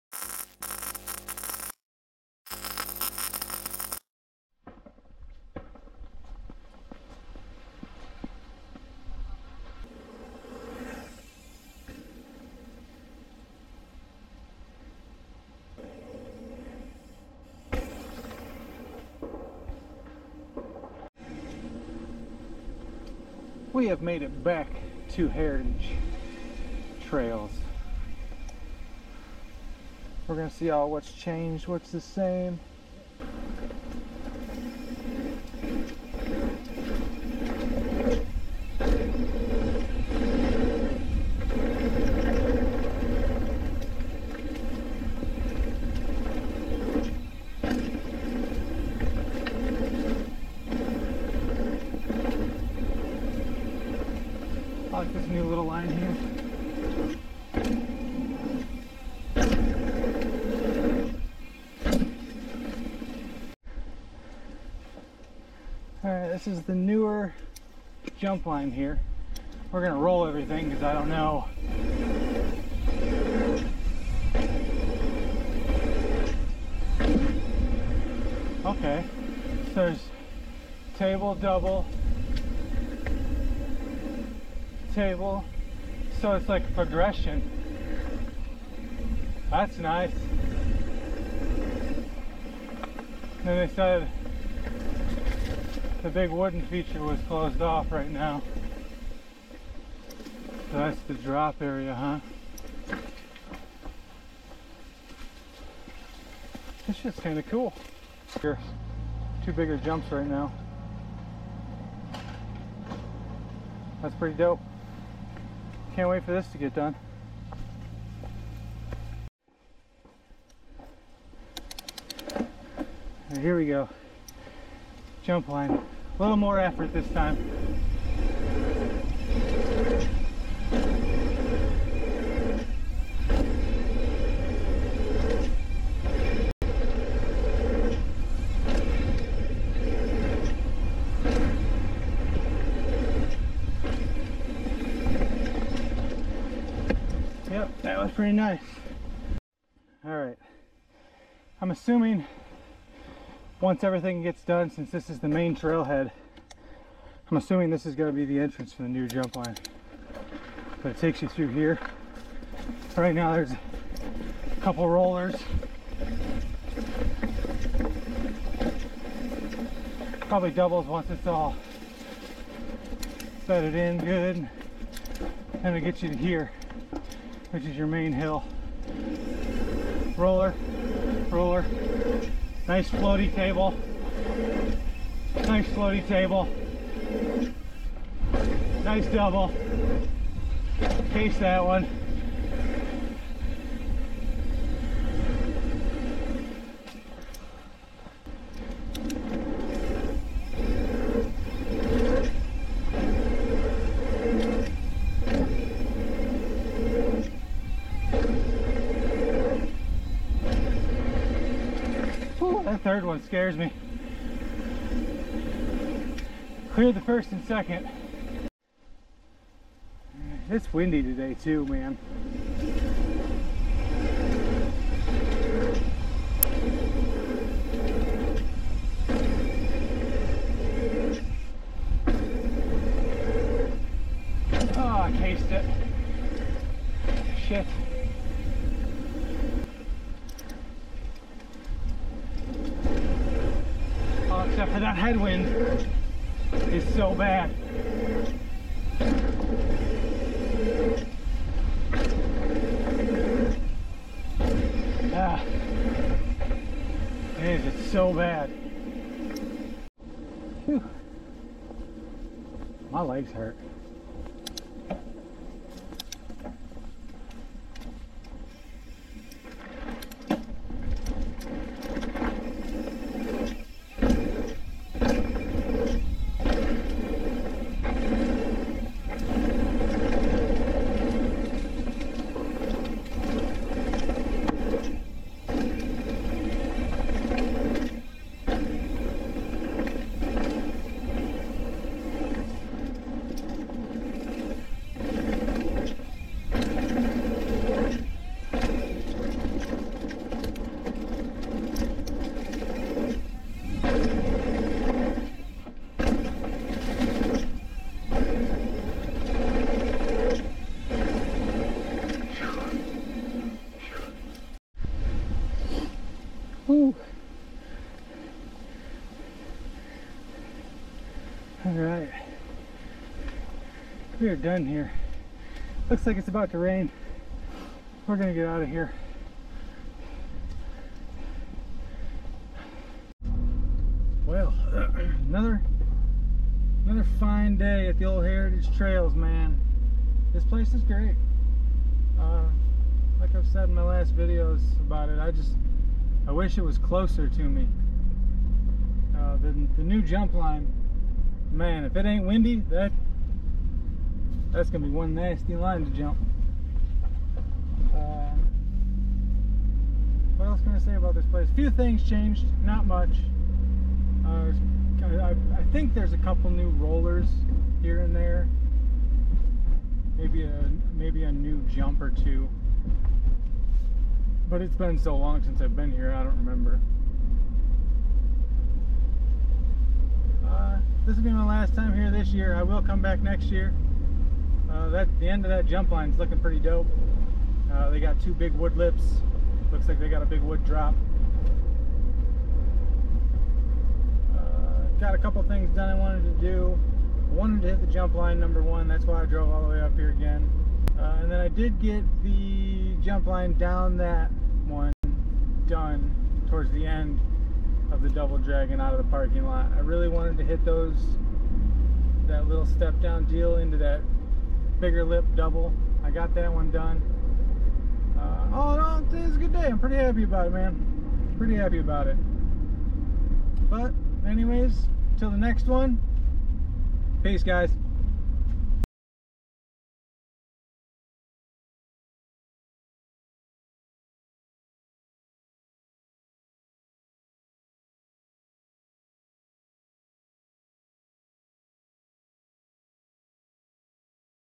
Heritage Trails | Jump Line | Trek Fuel EX7 | Jumps and Drops | Mtb Trails
About Heritage Trails Park
Metro's newest trails are still growing, and open during construction. Currently, there is 3.5 miles of singletrack and flow trail nearly complete, including several expert jumps and drops.

All three loops are bike-specific trails designed to help riders build their skills.

Double Dragon: nearly 1 mile of pure machine-built flow allows riders to safely develop their skill while offering a challenge to experienced riders. A- and B-lines allow riders to tailor the ride to their level -- soaring over jumps & drops or rolling smoothly around them
Take on Me: 1.7 miles of aggressive intermediate singletrack trail packed with berms, descents, climbs, rock gardens and even a couple optional jumps.
Fraggle Rocks: nearly 1 mile of smooth, mostly flat trail with optional rock gardens and roll-overs for new riders to tackle as they progress.
Future plans include a dedicated jump line, bike park, more singletrack and more flow.

Trail Status - check the Facebook Page
Amenities
Plenty of parking
Reservable picnic shelter
Portable toilets
Rules
All mountain bike trails are one-way, bike-only trails; bikes are not allowed on the hiking path

Trails will be closed when muddy

Always wear suitable protective equipment and study any feature before riding

Class 1 (pedal-assist) ebikes are welcome; throttle-controlled ebikes are prohibited - eBike class definitions can be found here

Access
Entry at 4668 County Hwy E, Slinger, WI 53086. Trailhead gateway is easily seen from the back parking lot.
All Washington County Parks have an access fee of $5/day per vehicle. Annual vehicle passes are available for residents ($30), non-residents ($40), or seniors & veterans ($20), and are good at all Washington County Parks.